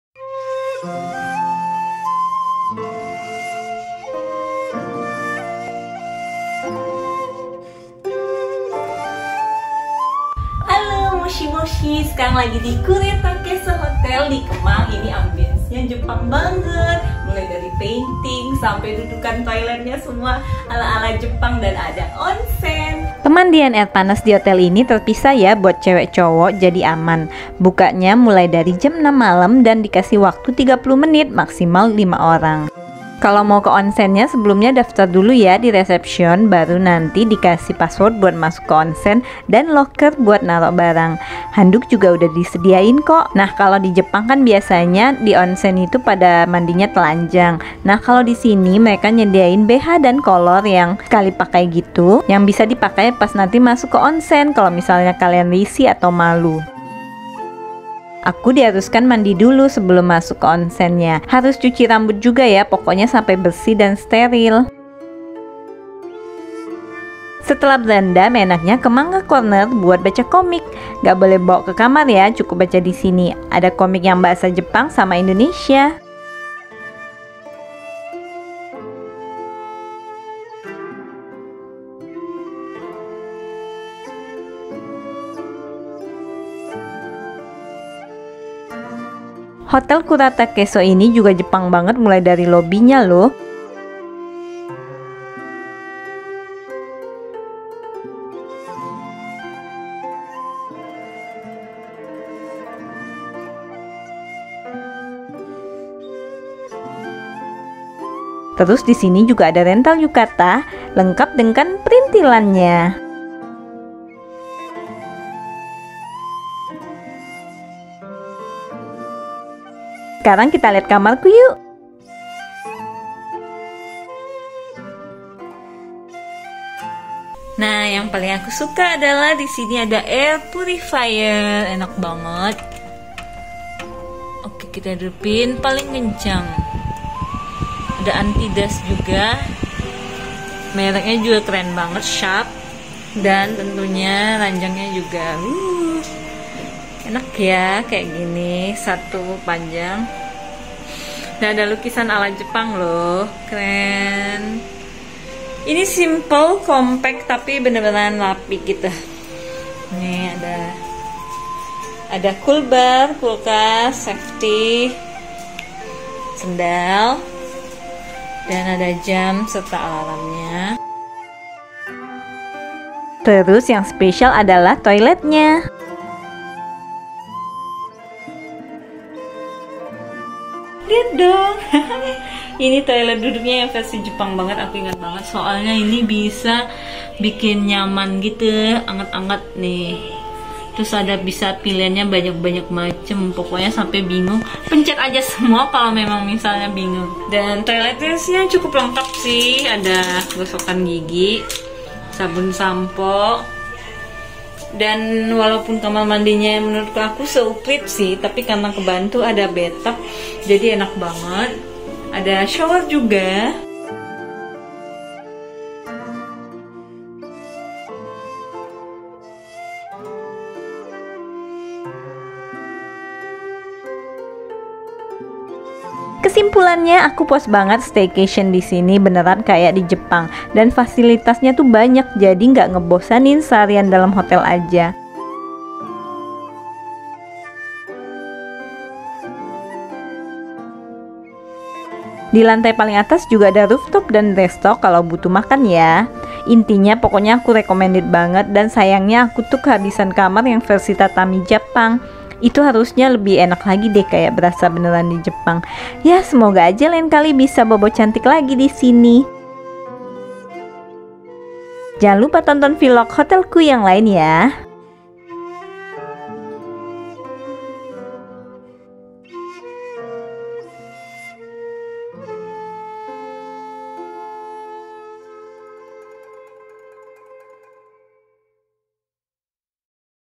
MANDI DI ONSEN HOTEL JEPANG DI KURETAKESO KEMANG JAKARTA!
Ada hotel Jepang keren loh di Kemang yang punya onsen! Namanya Kuretakeso dan fasilitas serta desain hotelnya Jepang banget!